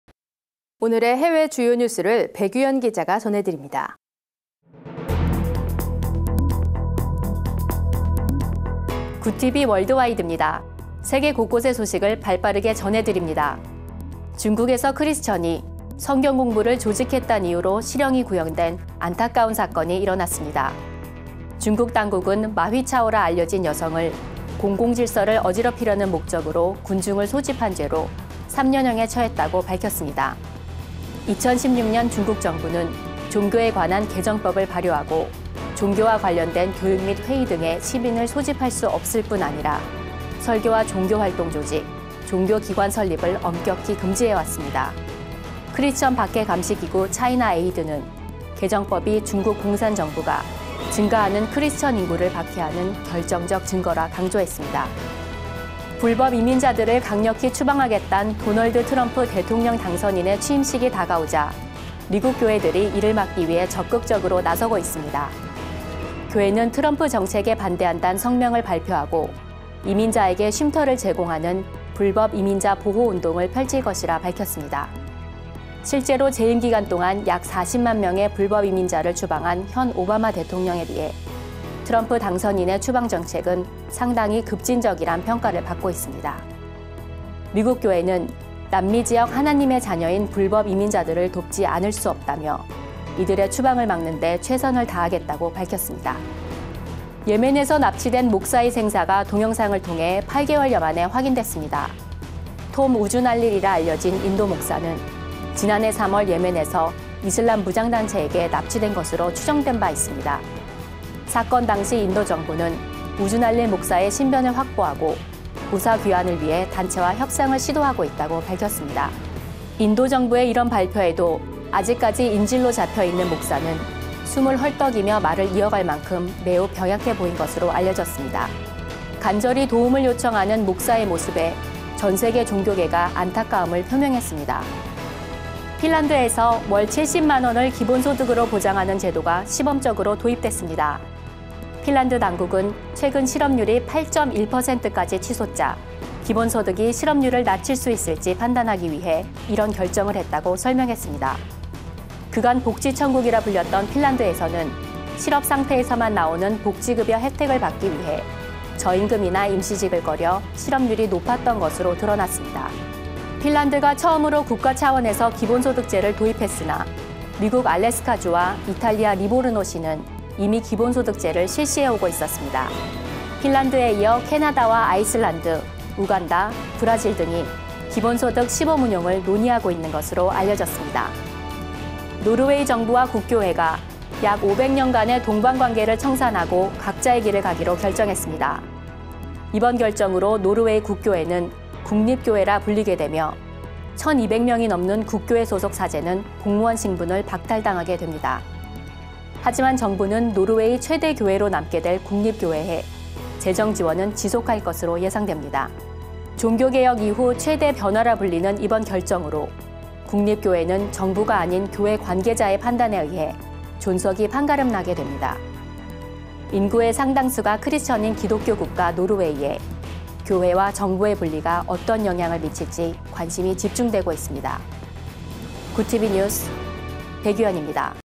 [GOODTV NEWS 20170104] GOODTV 월드와이드
오늘의 해외 주요 뉴스를 백유현 기자가 전해드립니다.

GOODTV 월드와이듭니다.
세계 곳곳의 소식을 발빠르게 전해드립니다.

1. 중국에서 크리스천이 성경공부를 개최했단 이유로 실형이 구형된 안타까운 사건이 일어났습니다. 중국 당국은 마 휘차오라 알려진 여성을 ‘공공질서를 어지럽히려는 목적으로 군중을 소집한 죄’로 3년 형에 처했다고 밝혔습니다. 2016년 중국 정부는 종교에 관한 개정법을 발효하고 종교와 관련된 교육 및 회의 등의 활동에 시민을 소집할 수 없을 뿐 아니라, 설교와 종교 활동 조직, 종교 기관의 설립을 엄격히 금지해왔습니다. 크리스천 박해감시기구 ‘차이나 에이드’는 개정법이 중국 공산 정부가 증가하는 크리스천 인구를 박해하는 결정적 증거라 강조했습니다.

2. 불법이민자들을 강력히 추방하겠단 도널드 트럼프 대통령 당선인의 취임식이 다가오자 미국 교회들이 이를 막기 위해 적극적으로 나서고 있습니다. 교회는 트럼프 정책에 반대한단 성명을 발표하고 이민자에게 쉼터를 제공하는 불법이민자보호운동을 펼칠 것이라 밝혔습니다. 실제로 재임기간 동안 약 사십만 명의 불법이민자를 추방한 현 오바마 대통령에 비해 트럼프 당선인의 추방 정책은 상당히 급진적이란 평가를 받고 있습니다. 미국 교회는 남미지역 하나님의 자녀인 불법 이민자들을 돕지 않을 수 없다며 이들의 추방을 막는데 최선을 다하겠다고 밝혔습니다.

3. 예멘에서 납치된 목사의 생사가 동영상을 통해 8개월여 만에 확인됐습니다. 톰 우주날릴이라 알려진 인도 목사는 지난 해 3월 예멘에서 이슬람무장단체에게 납치된 것으로 추정된 바 있습니다. 사건 당시 인도정부는 우주날릴 목사의 신변을 확보하고 무사귀환을 위해 단체와 협상을 시도하고 있다고 밝혔습니다. 인도정부의 이런 발표에도 아직까지 인질로 잡혀있는 목사는 숨을 헐떡이며 말을 이어갈 만큼 매우 병약해 보인 것으로 알려졌습니다. 간절히 도움을 요청하는 우주날릴 목사의 모습에 전세계 종교계가 안타까움을 표명했습니다.

4. 핀란드에서 월 70만원을 기본소득으로 보장하는 제도가 시범적으로 도입됐습니다. 핀란드 당국은 최근 실업률이 8.1까지 치솟자 기본소득이 실업률을 낮출 수 있을지 판단하기 위해 이런 결정을 했다고 설명했습니다. 그간 복지 천국이라 불렸던 핀란드에서는 실업상태에서만 나오는 복지급여 혜택을 받기 위해 저임금이나 임시직을 꺼려 실업률이 높았던 것으로 드러났습니다. 핀란드가 처음으로 국가 차원에서 기본소득제를 도입했으나, 미국 알래스카 주와 이탈리아 리보르노 시는 이미 기본소득제를 실시해오고 있었습니다. 핀란드에 이어 캐나다와 아이슬란드, 우간다, 브라질 등이 기본소득 시범 운용을 논의하고 있는 것으로 알려졌습니다.

5. 노르웨이 정부와 국교회가 500여 년간의 동반관계를 청산하고 각자의 길을 가기로 결정했습니다. 이번 결정으로 노르웨이 국교회는 국립교회라 불리게 되며, 1200명이 넘는 국교회 소속 사제는 공무원 신분을 박탈당하게 됩니다. 하지만, 정부는 노르웨이 최대의 교회로 남게 될 국립교회에 재정지원은 지속 할 것으로 예상됩니다. ‘종교개혁 이후 교회 최대의 변화’라 불리는 이번 결정으로 국립교회는 정부가 아닌 목회자를 비롯한 교회 관계자의 판단에 의해 존속이 판가름 나게 됩니다. 인구의 상당수가 크리스천인 기독교국가 노르웨이에 교회와 정부의 분리가 어떤 영향을 미칠지 관심이 집중되고 있습니다.

GOODTV NEWS 백유현 입니다.

* 시청방법
- IPTV: KT Olleh TV 234번, SKB Btv 303번
- 케이블: 티브로드 224번, CJ헬로비전 703번, 딜라이브(씨앤앰) 702번, HCN(서울) 545번